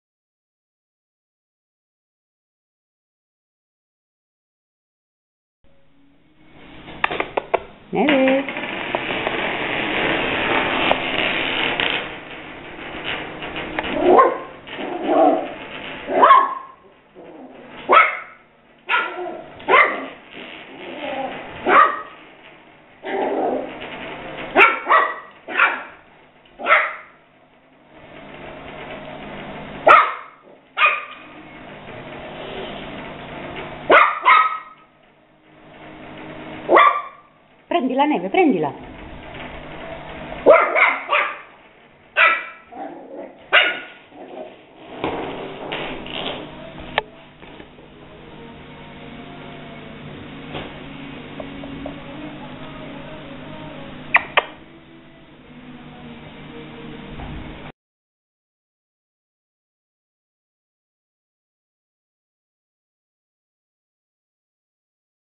Neve e la trottola
La cucciolotta fa conoscenza con la versione "antica" dei moderni beyblade!